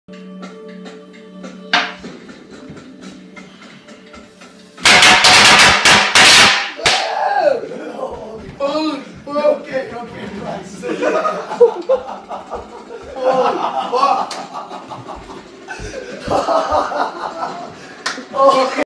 never fall asleep
When you're always the first one to fall asleep, eventually something like this will happen.